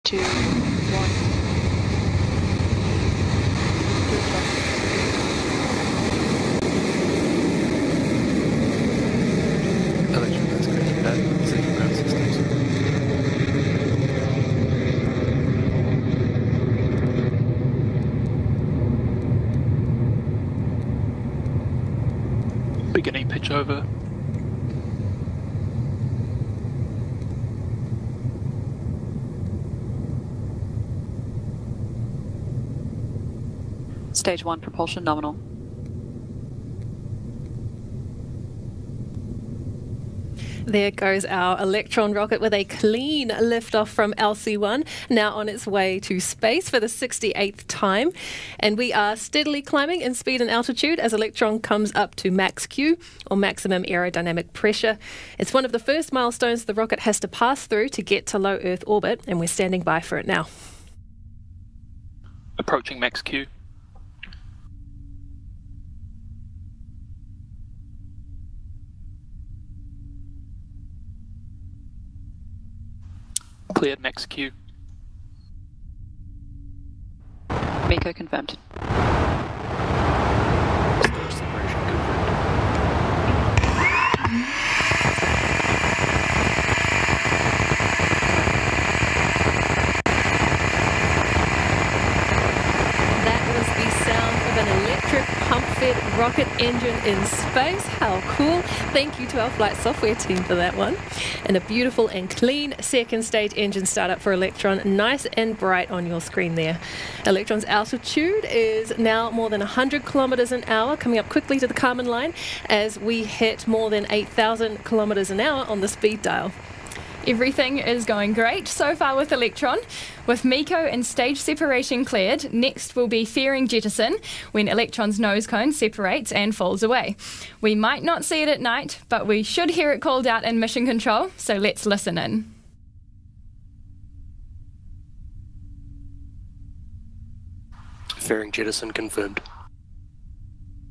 Watch This Rocket Blast Off Into Space!
First of two launches, sending a singular spacecraft to a 650km circular orbit for a confidential commercial customer.

The launch date was Saturday, June 28, 2025 at 7:08 AM (UTC).